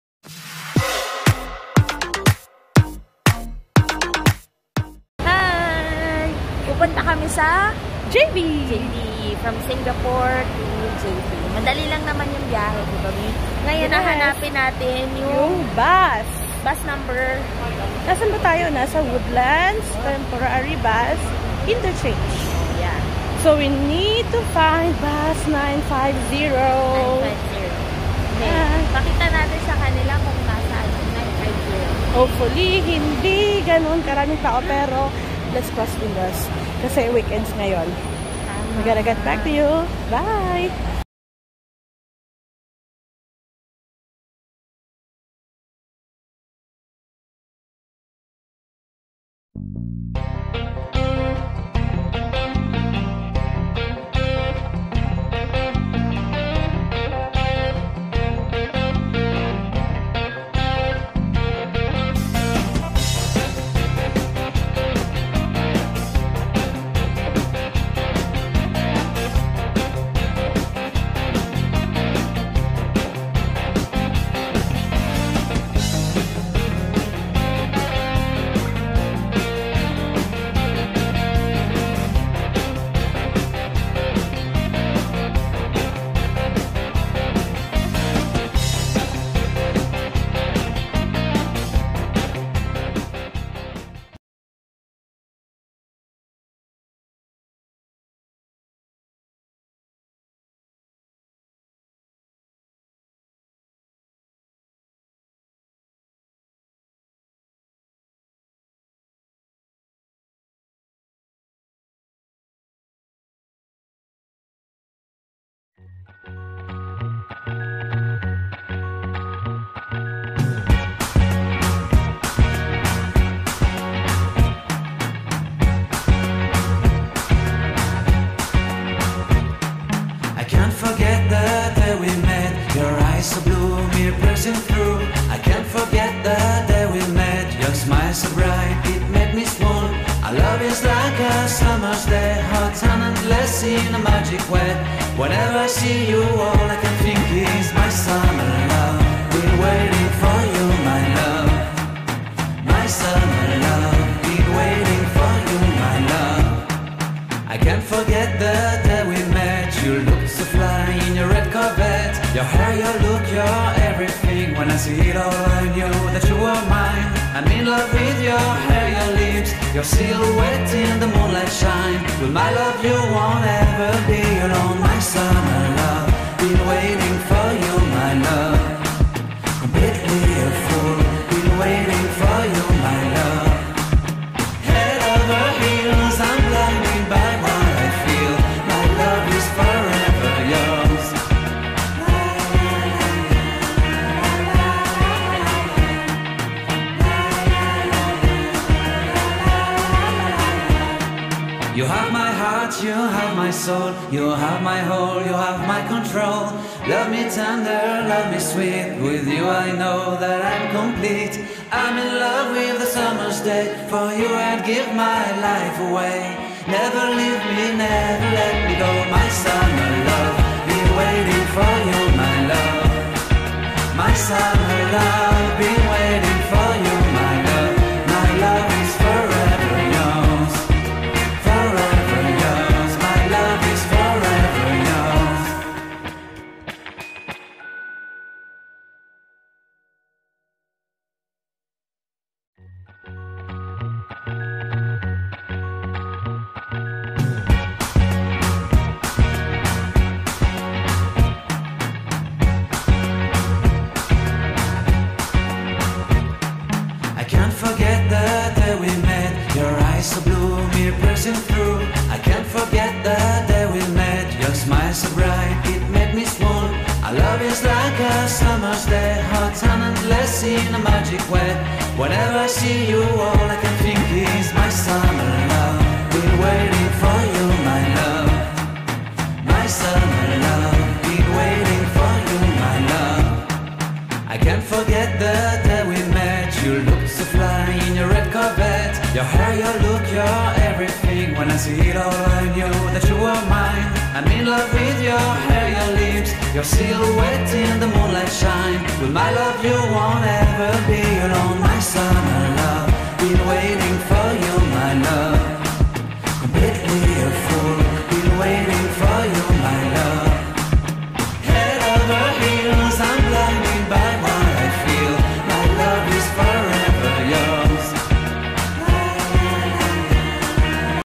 How to get to Johor Bahru, Malaysia from Singapore via Woodlands Checkpoint
Bus Trip from Singapore to JB, Malaysia

Hi guys, do you know that we can travel from Singapore to Johor Bahru, Malaysia via land transport(bus)?

Johor Bahru and Woodlands is well-known for being neighbouring towns of both countries.

In this video, we head to Woodlands Temp Bus Interchange, riding bus 950, to reach Johor Bahru, Malaysia. Travel time will vary upon schedule (heavy loads for weekends and holidays). Locals and travellers may use their SMRT cards to pay for the bus fair or coins for tickets upon riding the bus. Price may vary  at $1 to $2 (sgd).

For other options, you may try a shuttle service (Causeway Link Bus) at Kranji Mrt Station.

Upon reaching Woodlands Checkpoint, clear  your travel documents by scanning or presenting it to an immigration officer. Once done, find the bus platform and ride for the next stop Malaysia Immigration(Customs), JB Checkpoint (10-15mins, may vary upon traffic). Clear travel documents and Voila!, you have reach your destination.

Across JB Checkpoint is JB Sentral City Square Mall where you can chill and relax for awhile, it offers shopping botiques, stalls, kids zone and restaurants that are really way cheaper than we expected 😊😊😊. You may also like to check out Komtar Mall beside it.

For hailing cabs to your hotel, you may want to book a grab instead of local taxis (teksi), because it is cheaper. Always remember to change your mode of payment in CASH instead of CARD as you will pay based on your card's currency even when the app price shows malaysian ringgit (10malaysian ringgit (3sgd) will be 10sgd if you pay by card).

TIP:
If you are at JB City Square, hail a grab at Levi's Store as your pick-up point. It's at the ground floor and very east to locate. (Short video clip is here)

Weather is really hot and humid especially during high-noon

At evening, you may enjoy night market, carnival resto-bars around town.

Voila! Now, you are set to explore. Happy travelling 😊😊😊

Viajera Urbano